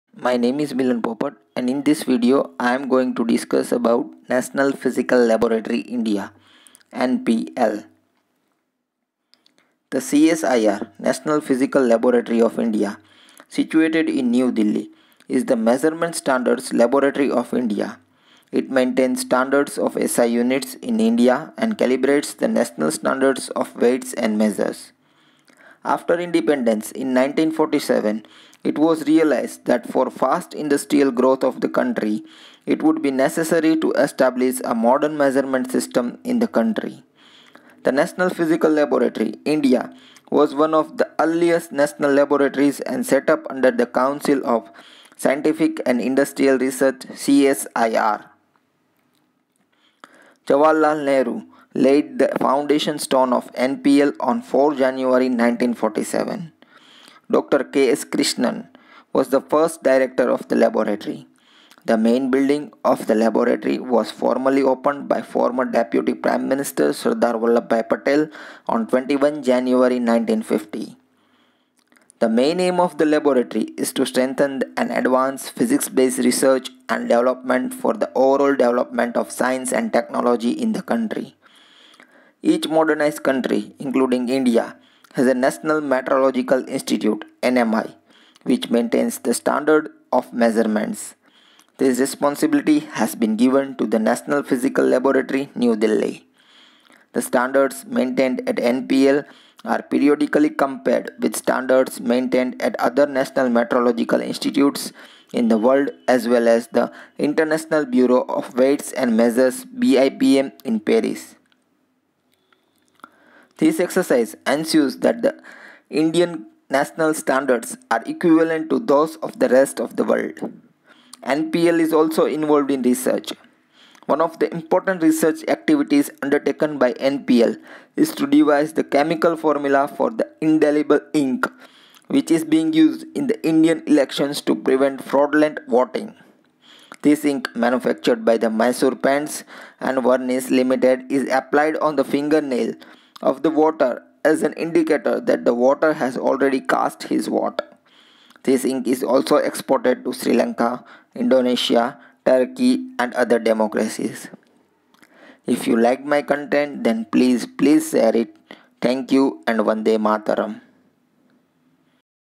National Physical Laboratory-India
In this video, a short introduction and research work of National Physical Laboratory-India is Discussed.